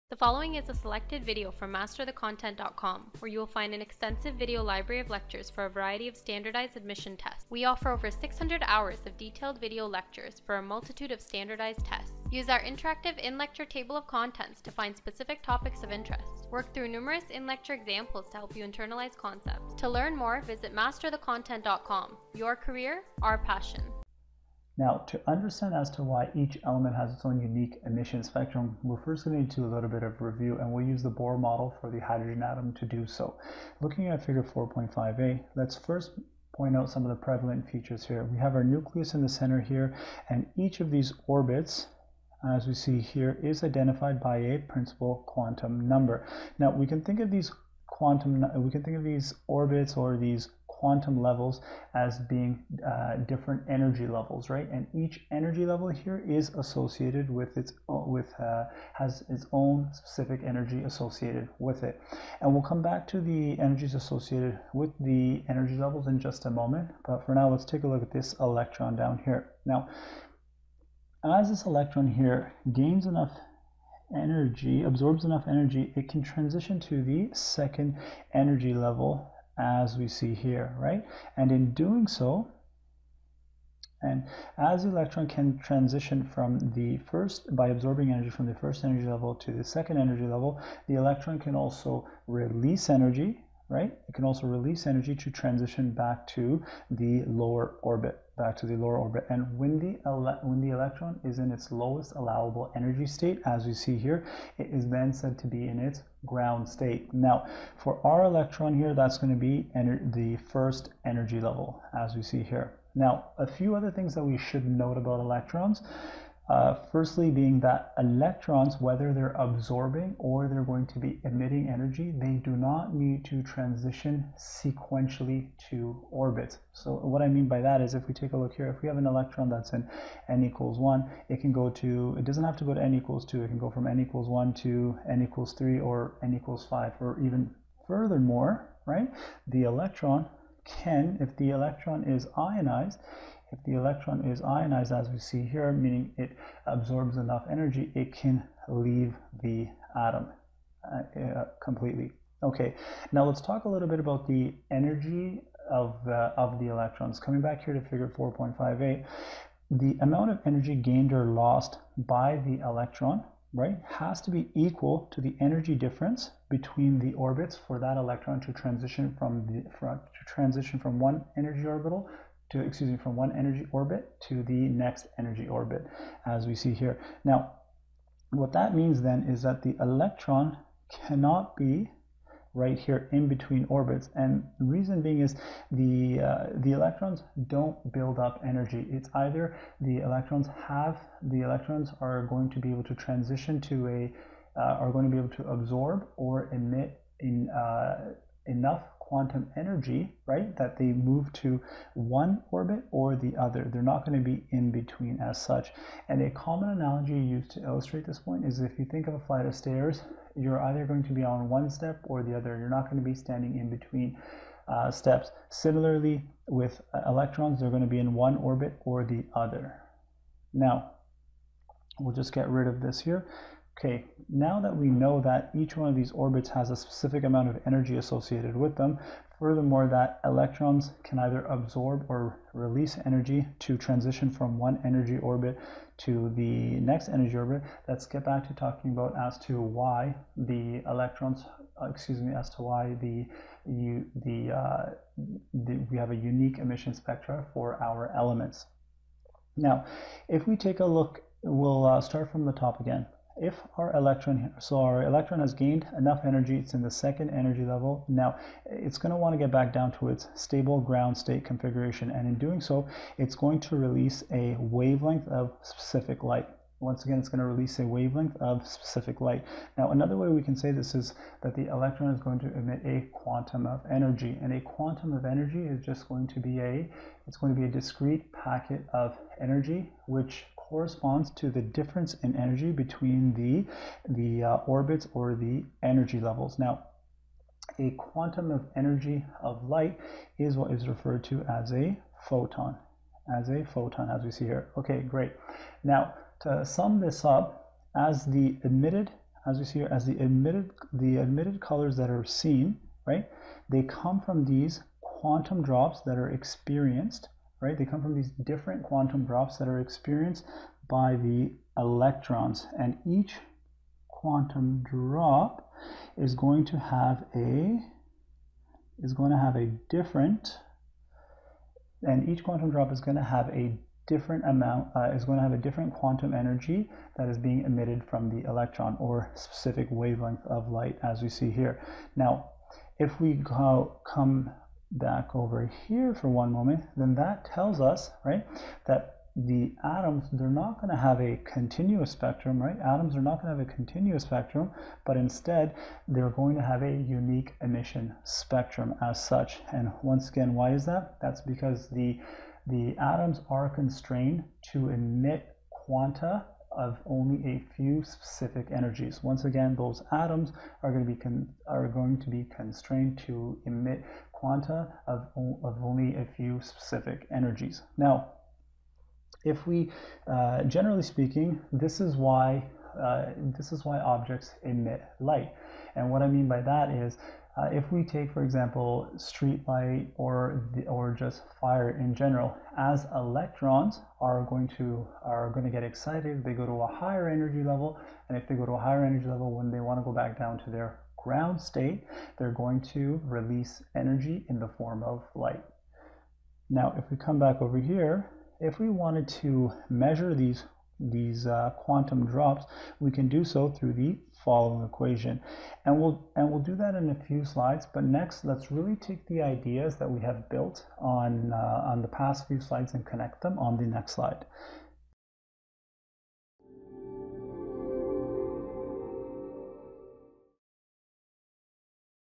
MCAT®: Emission Line Spectra Explained
► Medical College Admission Test | Chemical and Physical Foundations of Biological Systems | Content Category 4E: Atoms, Nuclear Decay, Electronic Structure, and Atomic Chemical Behavior | Electronic Structure Part 1

 ► Lecture Description:

- This lecture focuses on electronic structure. Topics covered: electromagnetic spectrum, frequency (+ equation), wavelength, amplitude, atomic line spectra (continuous spectrum, line spectrum, emission spectrum, white light), Bohr Model for hydrogen atom, energy absorption by an electron, excited state and ground state of an electron, energy of a photon (+ equation), Planck’s constant, energy and wavelength equation, Balmer series, Lyman series, Paschen series, Balmer-Rydberg equation, and De Broglie Equation.